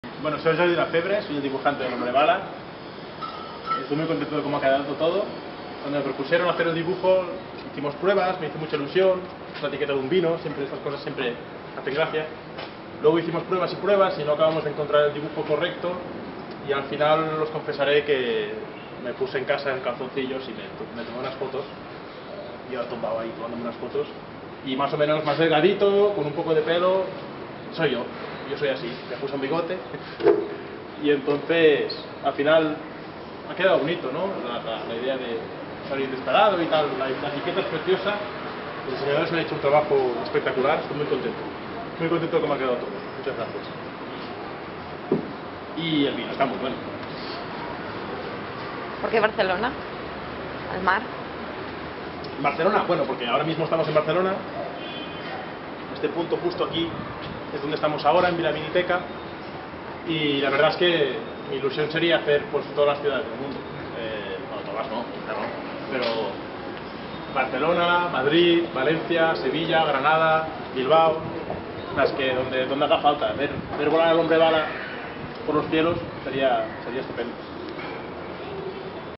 Entrevista a Jordi Lafebre, ilustrador de El Hombre Bala
Jordi Lafebre, ilustrador de El Hombre Bala, entrevistado después de dibujar el escaparate de Vila Viniteca.